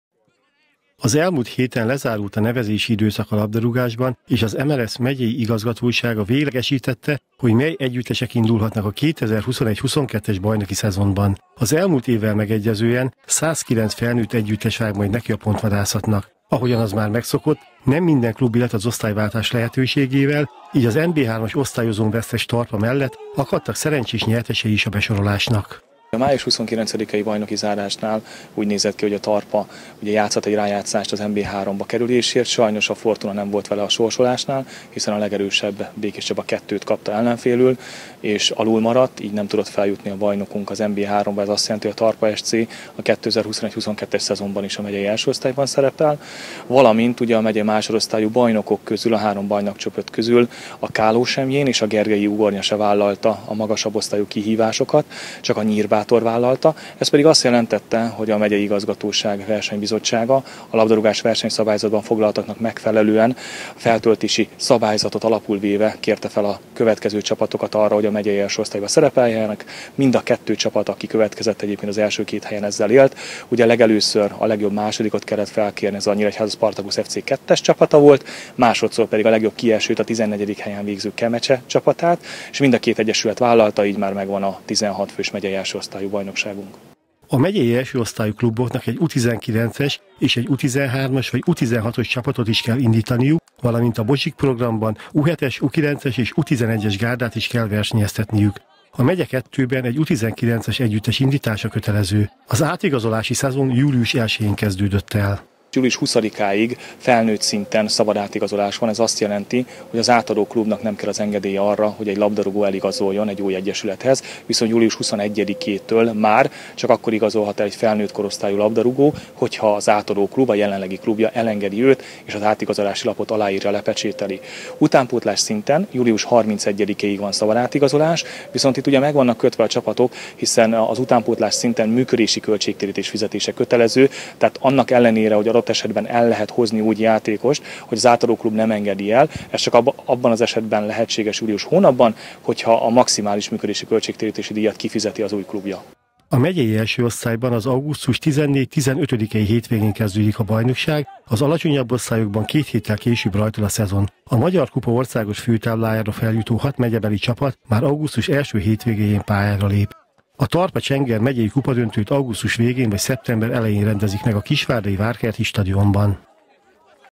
109 csapat nevezett
Százkilenc felnőtt csapat részvételével kezdődik majd el Szabolcs-Szatmár-Bereg megyében a 2021/22-es futballszezon.
A megyei első osztályban tizenhat csapattal augusztus közepén rajtol a bajnokság, a három tizennégy csapatos másodosztályú csoportban és a harmadosztályban augusztus utolsó hétvégéjén indul útjára a labda.